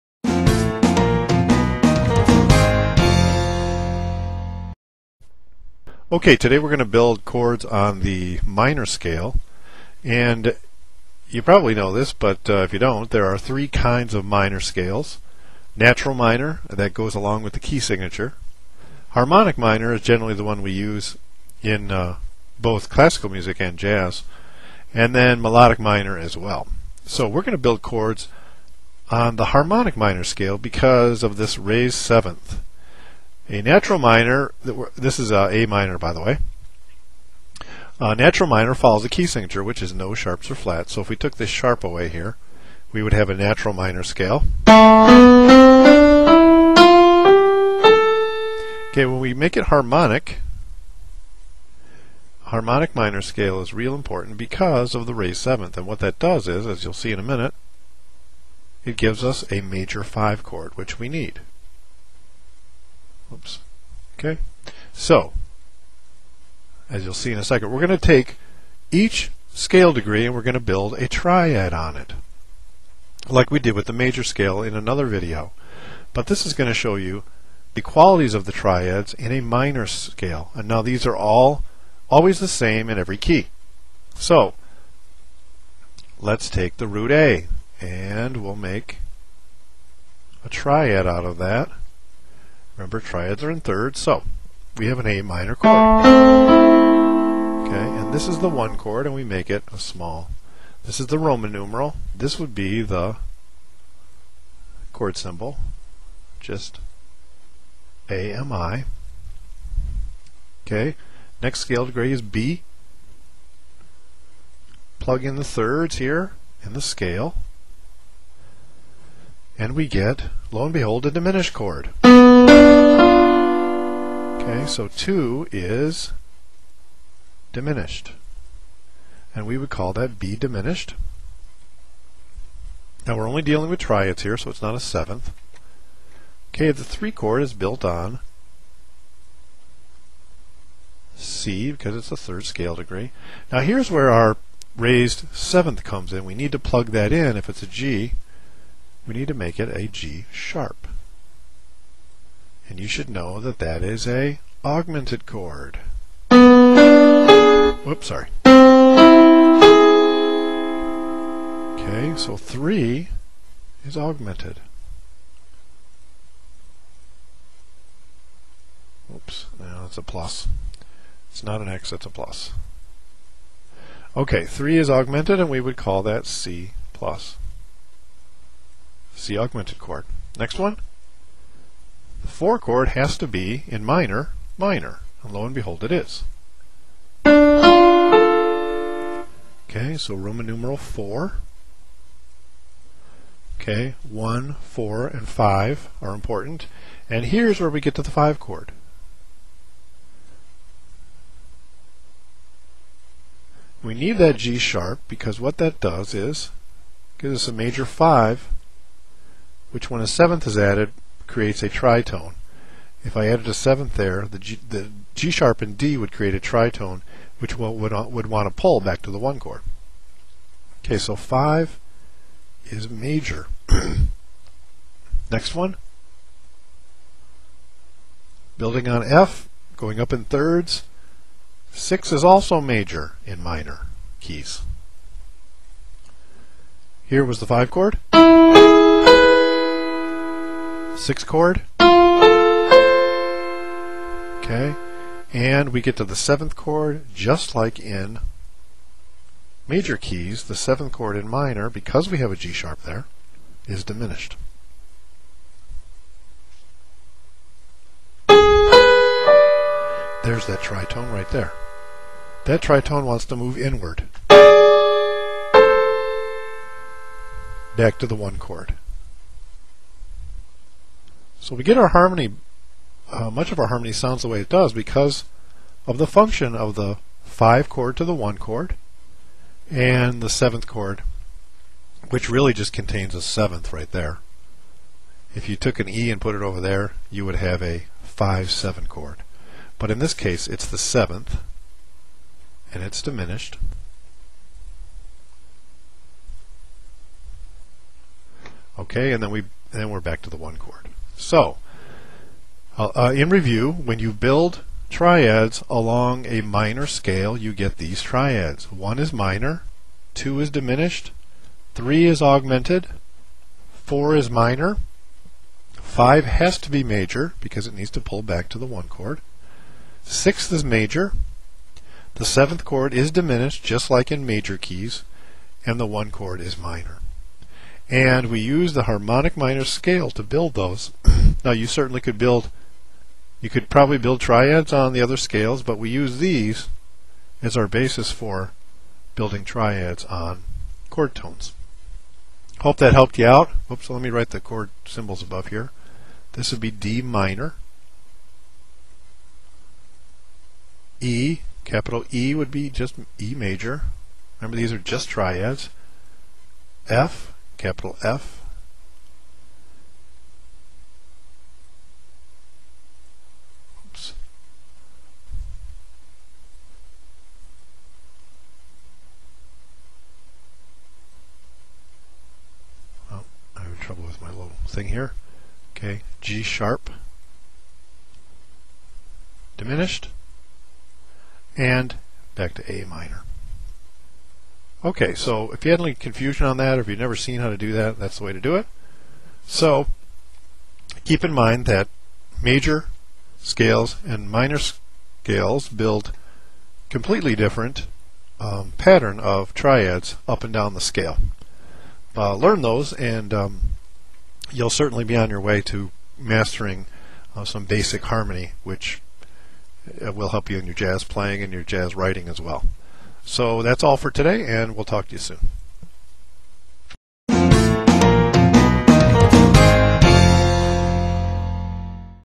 Building Triads on the Harmonic Minor Scale
Just as triads can be built on the tones of a major scale, they can be constructed on the scale degrees of minor scales as well. There are 3 different minor scale forms - natural minor, harmonic minor and melodic minor. All three forms are used in jazz playing and in jazz writing.

Minor Scale Refresher

Natural minor scale = follows the key signature straight up. The natural minor scale in the key of A minor would subsequently follow the key signature (no sharps or flats) straight up from A to A.

Harmonic minor scale = raising the 7th scale degree of the natural minor. In the case of A harmonic minor, a G now becomes a G# in the scale. This alteration gives the scale a completely different sound. We need this raised 7th to form a major V chord and to help give the key its tonality.

Melodic minor scale = raised 6th and 7th going upwards (in A minor it would include F# and G# going up) and then natural minor scale coming downwards.

This video shows the qualities of triads that can be built on the steps of the harmonic minor scale. Notice how different the qualities of chords are as triads are built on each step of this scale.

Got a combo, big band, or interested in learning jazz arranging?  Jazz arranging tutorials too!